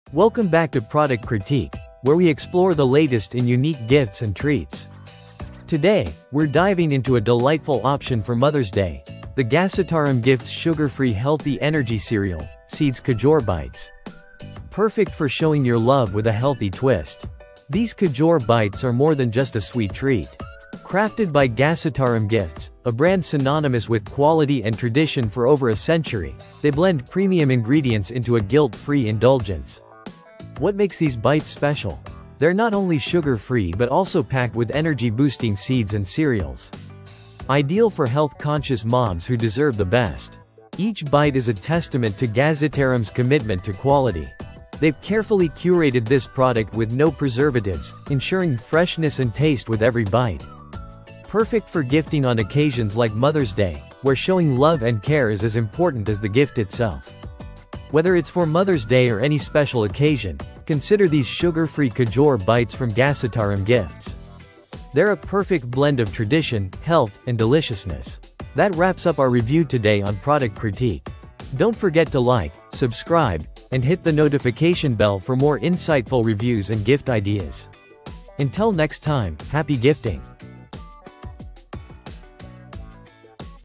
🍏 Ghasitaram Gifts Mother's Day Gifts Sugarfree Healthy Bites | Best Sugar Free Mothers Day Gifts 🎁
Amazon Product Name : Ghasitaram Gifts Mother's Day Gifts Sugarfree Healthy Energy Cereal/Seeds Khajoor Bites

In this video, we showcase the "Best Sugar Free Mother's Day Gifts," perfect for celebrating the special moms in your life while keeping their health in mind. From delicious treats to thoughtful presents, these gifts are both sweet and considerate. Join us as we explore the "Best Sugar Free Mother's Day Gifts," highlighting unique ideas and options that show your love without the added sugar!